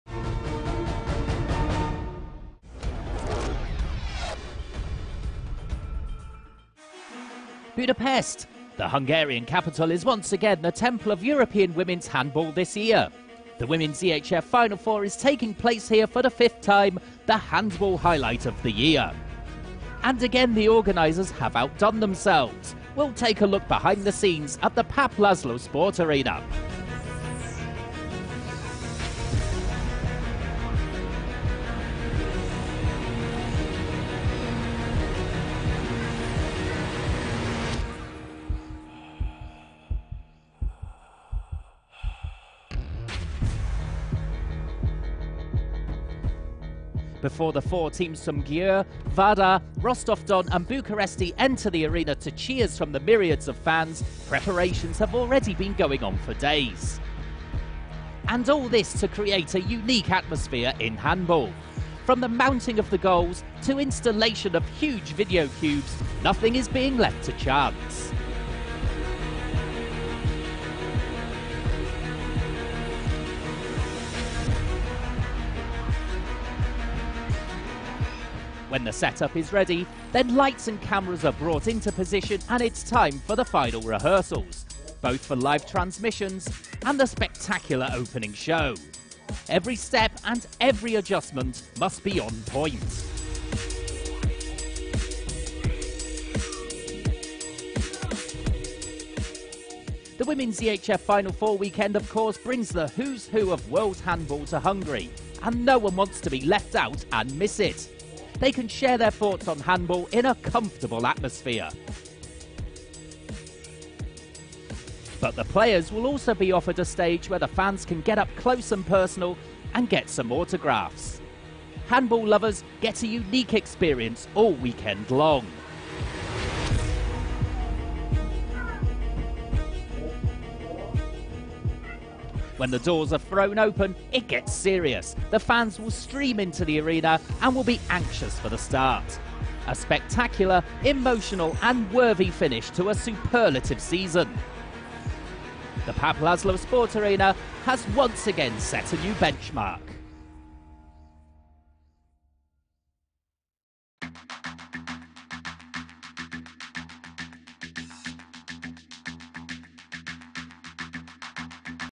Closer Look | Behind the scenes | Women's EHF Champions League 2017/18
Budapest, was the temple of European Women handball this year. Let's take a look behind the scenes of what happend last weekend.